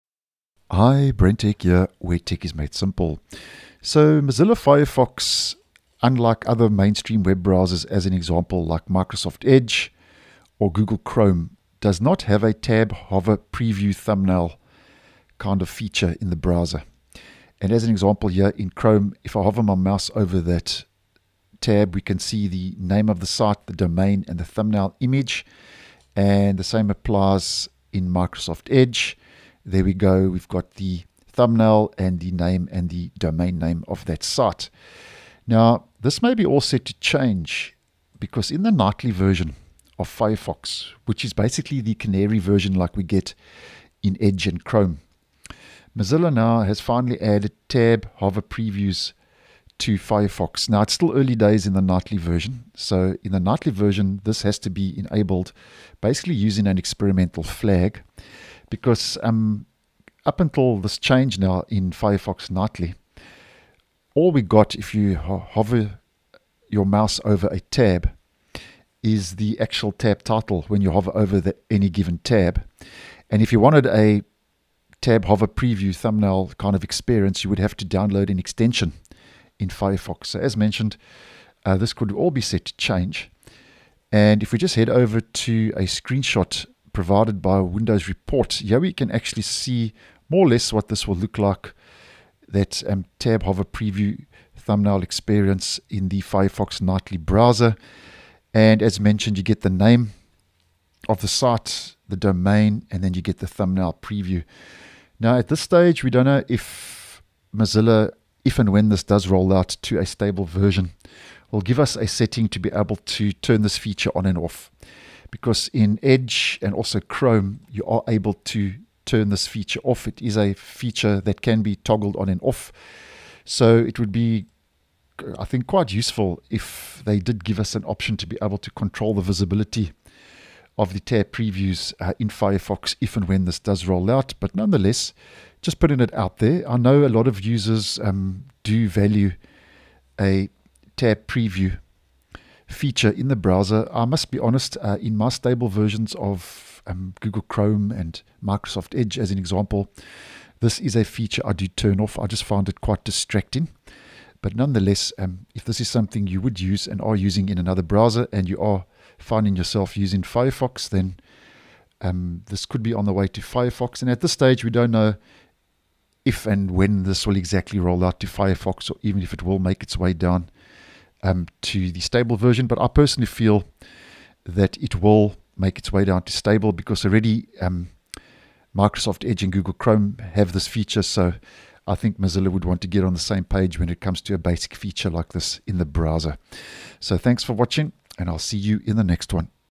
Mozilla Firefox will Finally support Tab Hover Previews
As posted by windowsreport Mozilla has finally added Tab Hover Previews support to Firefox in the Nightly version.